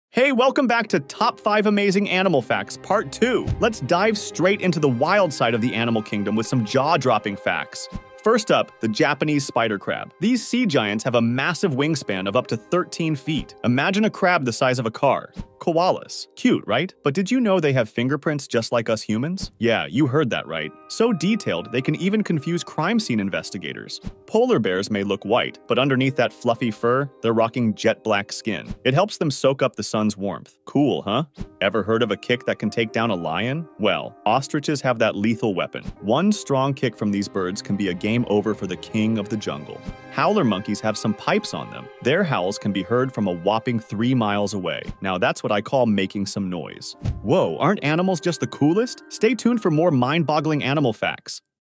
5 Amazing Animal Facts Part 2
"🐾 Dive into the wild wonders of nature with FIVEWILDEST! 🌿 In our latest installment, discover 5 astonishing animal facts that will leave you in awe! From the incredible adaptations of deep-sea creatures to the surprising behaviors of land-dwelling marvels, this video is packed with jaw-dropping moments you won't want to miss! 🦁🐬 AnimalFacts Nature Wildlife"